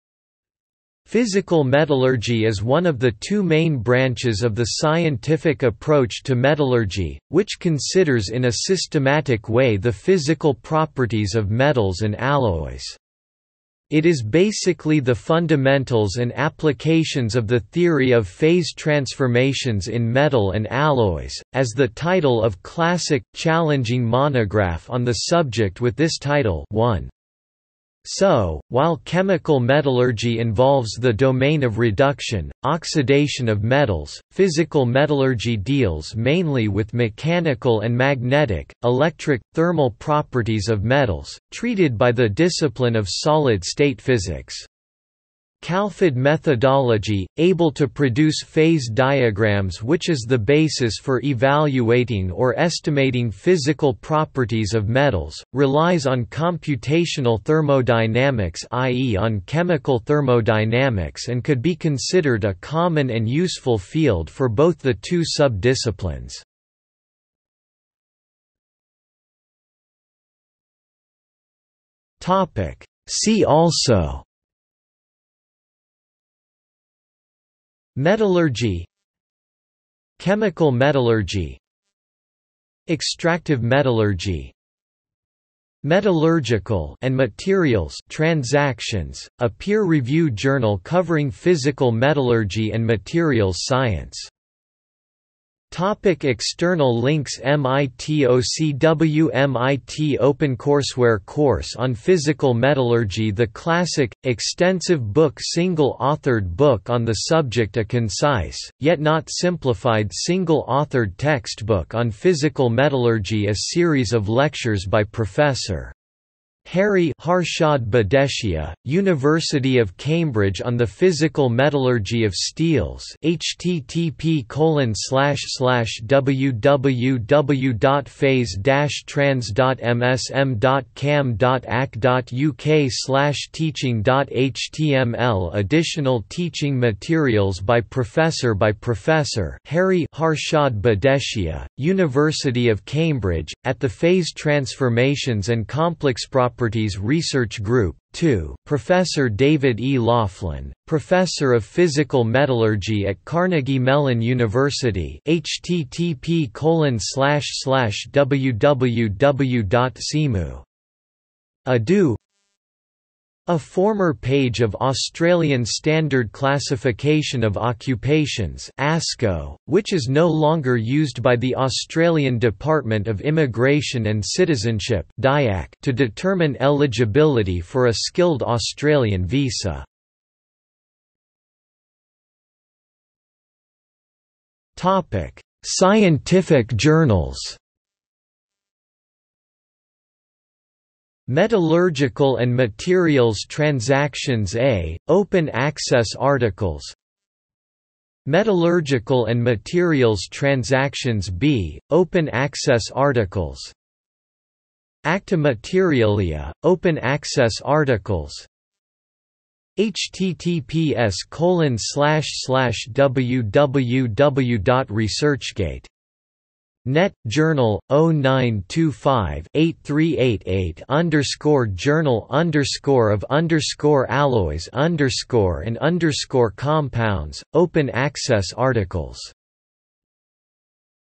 Physical metallurgy | Wikipedia audio article
00:01:08 See also
00:01:35 External links
00:03:01 Scientific Journals

- Socrates

SUMMARY
Physical metallurgy is one of the two main branches of the scientific approach to metallurgy,  which considers in a systematic way the physical properties of metals and alloys. It is basically the fundamentals and applications of the theory of phase transformations in metal and alloys, as the title of classic, challenging monograph on the subject with this title  [1]. So,  while chemical metallurgy involves the domain of reduction/oxidation of metals, physical metallurgy deals mainly with mechanical and magnetic/electric/thermal properties of metals – treated by the discipline of solid state physics. Calphad methodology, able to produce Phase diagrams which is the basis for evaluating or estimating physical properties of metals, relies on Computational thermodynamics i.e. on Chemical thermodynamics and could be considered a common and useful field for both the two sub-disciplines.